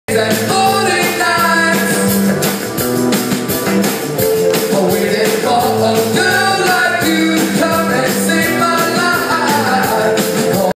Fitz and the Tantrums at SXSW 2013
Fitz and the Tantrums performs at KGSR's live broadcast at the W Hotel during SXSW 2013. Shot by Preston Jones of DFW.com.